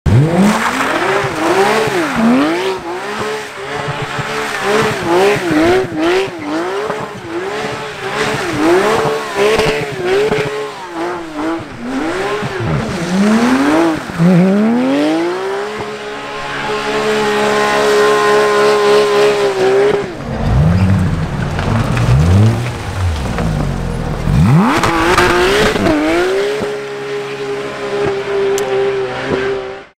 Porsche 911 GT3 RS donuts on Bergen Cannonball Run
Produsert av Cannonball AS.

Alle rettigheter, uavhengig av distribusjonskanal, herunder YouTube, eies i sin helhet av Cannonball AS.